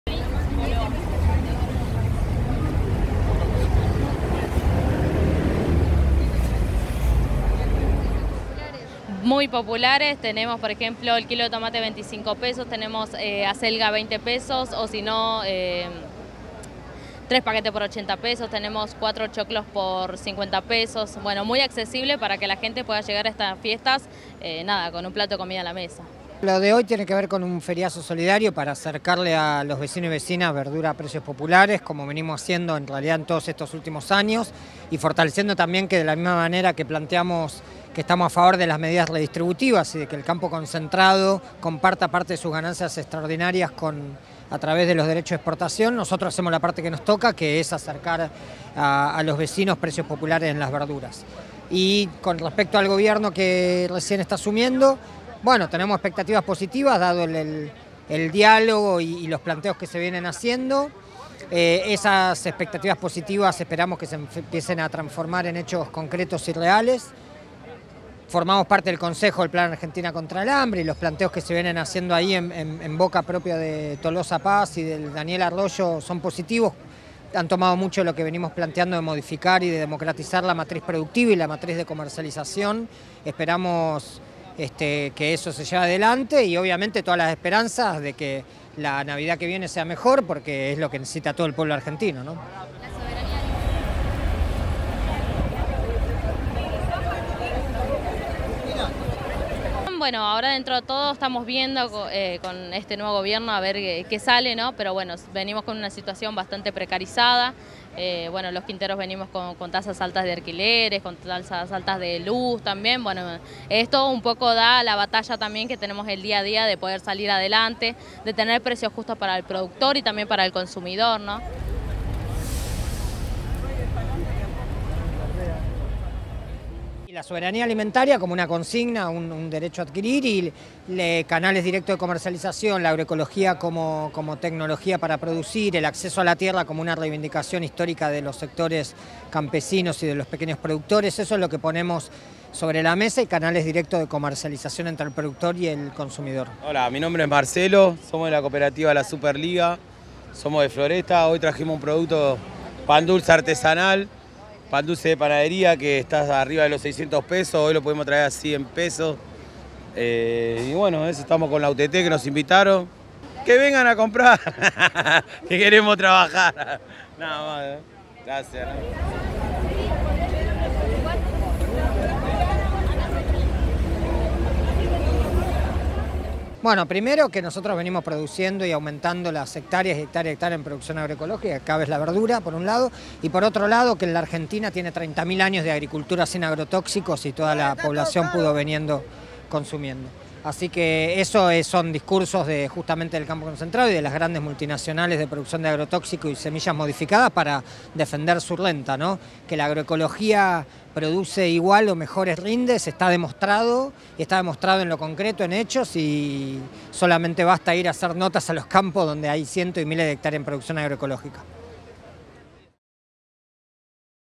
Feriazo Navideño de la UTT
En la Plaza Once, de la ciudad de Buenos Aires, los pequeños productores de la UTT (Unión de Trabajadores de la Tierra) realizaron un feriazo navideño para a acercar verduras y frutas a precios populares, y que en estas fiestas todas las familias accedan a un plato de comida.
También en Mendoza y Misiones se realizan hoy "verdurazos" navideños. Y hubo Feria en el Mayorista de Avellaneda: Lamadrid 758, Dock Sud.
La verdura a precios populares es posible porque al vender de forma directa no existen intermediarios que aumentan hasta un 100% el valor de los alimentos. La propuestas de los pequeños productores al nuevo gobierno.